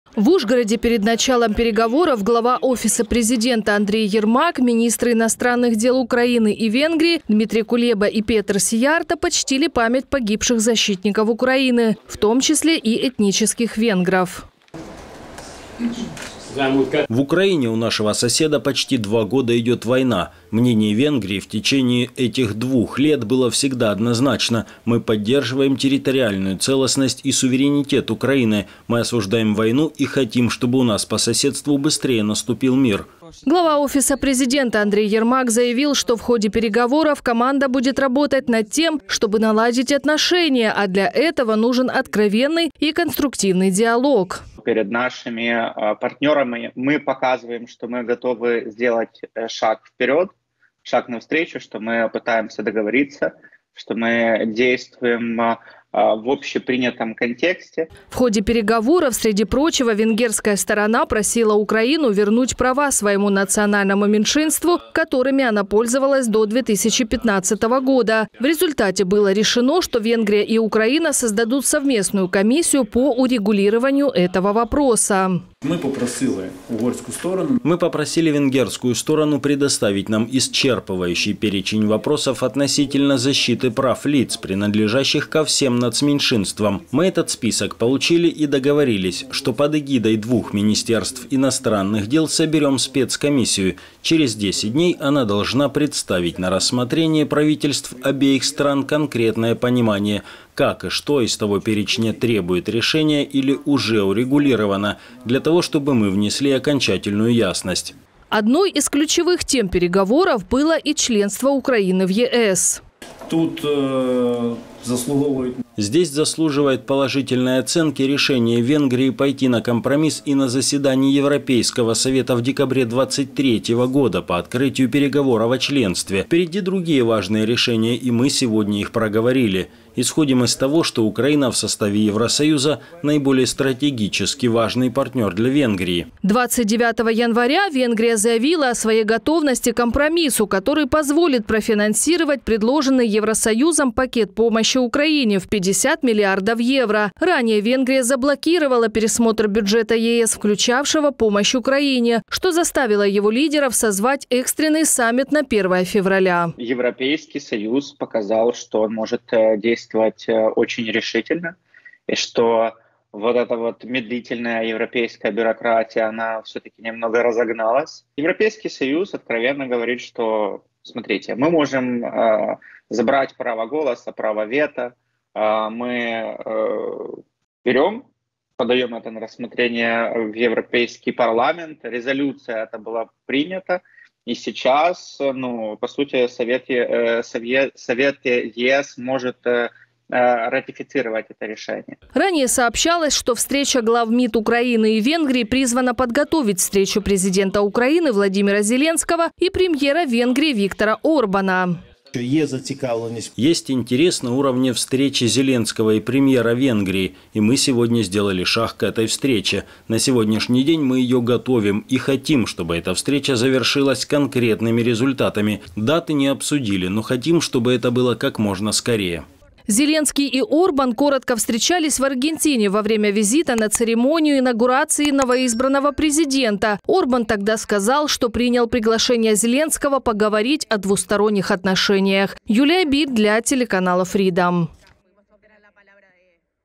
⚡️ ПЕРЕГОВОРЫ в Ужгороде: о чем договорились СИЙЯРТО, ЕРМАК и КУЛЕБА?
Руководитель Офиса президента Украины Андрей Ермак и глава МИД Дмитрий Кулеба накануне в Ужгороде встретились с министром иностранных дел и внешней торговли Венгрии Петером Сийярто. Стороны обсудили нормализацию отношений между Киевом и Будапештом, а также усиление диалога на уровне общин. Переговоры длились более шести часов и, как отметил Андрей Ермак, подтвердили взаимное стремление Венгрии и Украины провести встречу на высшем уровне.

О чем еще удалось договориться, смотрите в материале журналистов FREEДОМ.